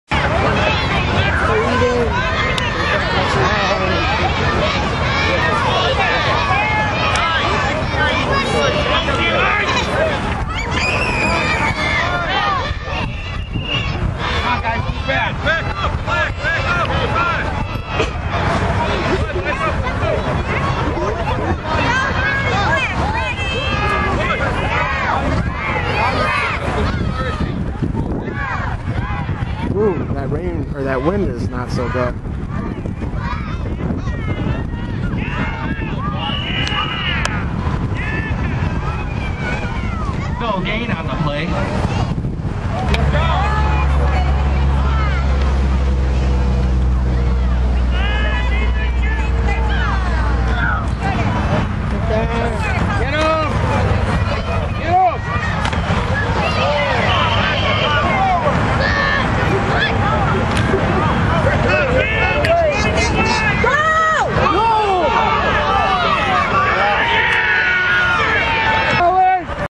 Eden Raiders Pee Wee football
9/30/2011 Eden Raiders vs CSRA pee wee little loop football CSRA  part 12